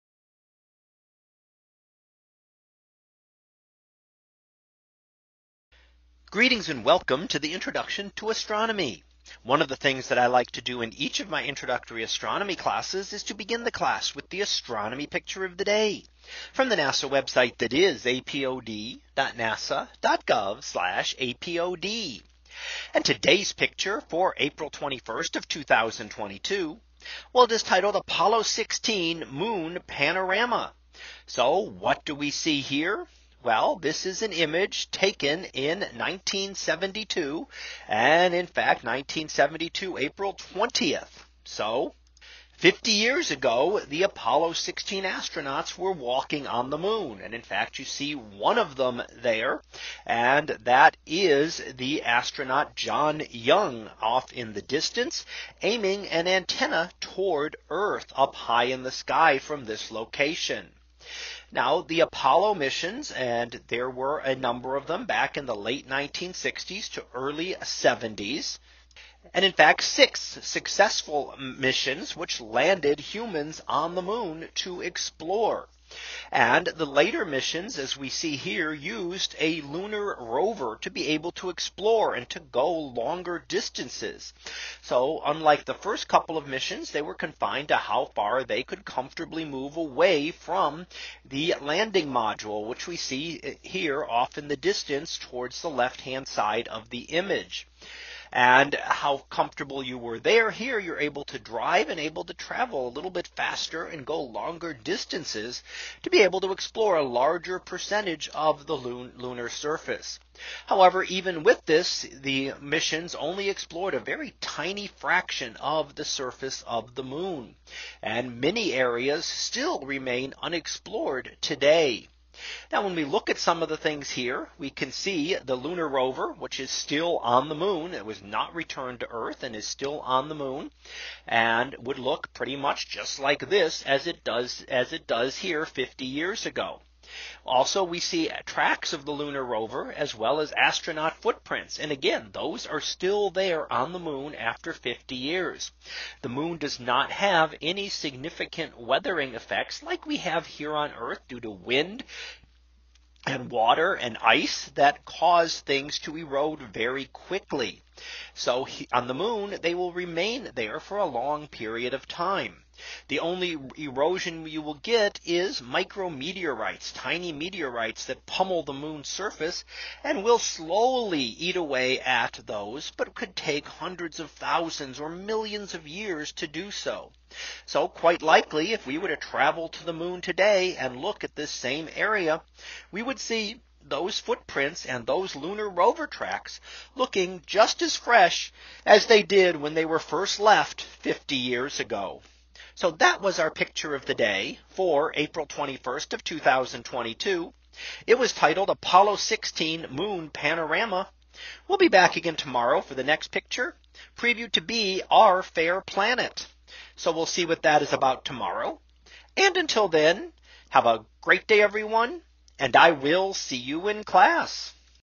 2022 April 21 - Apollo 16 Moon Panorama
In today's image, we see an image of the Moon put together into a panorama. This is from the Apollo 16 mission which landed on the Moon 50 years ago yesterday.